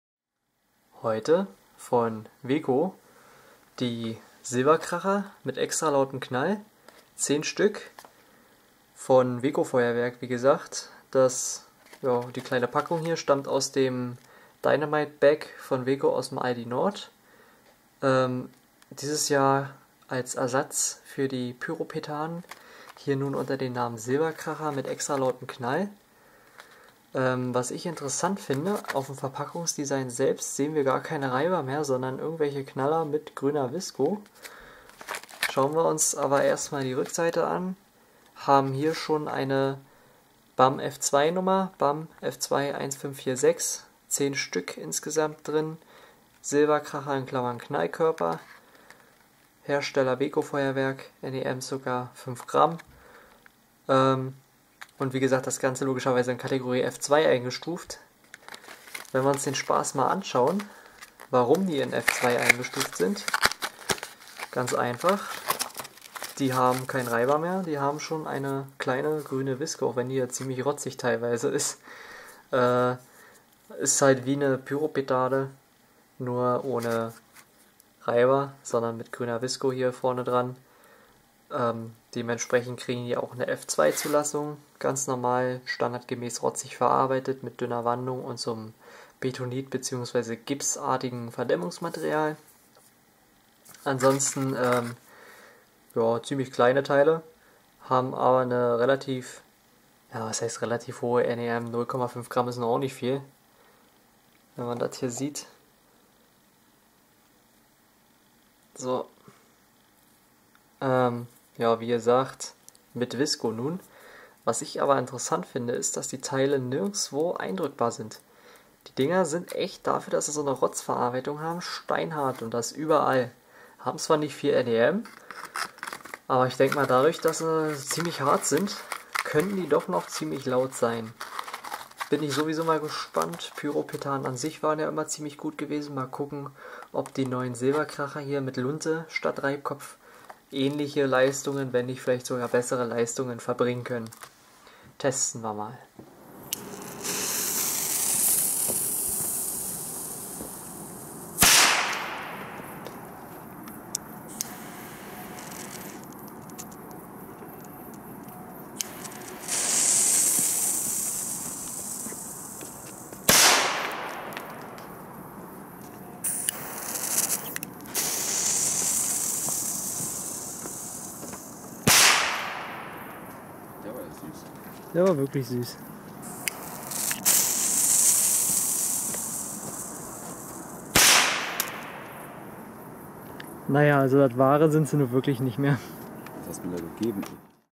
Weco Silberkracher (mit extra lautem Knall)
Die neue Version der Pyro Petarden von Weco, unter dem Namen Silber Kracher mit grüner Visco. Immer noch ganz nett, aber nicht mehr wirklich das gelbe vom Ei. Für mich als Reiberfan etwas enttäuschend, wegen der F2 Zulassung wohl aber nicht anders möglich gewesen...

Klasse 2
Knallkörper/Böller
Preis: ~0,20€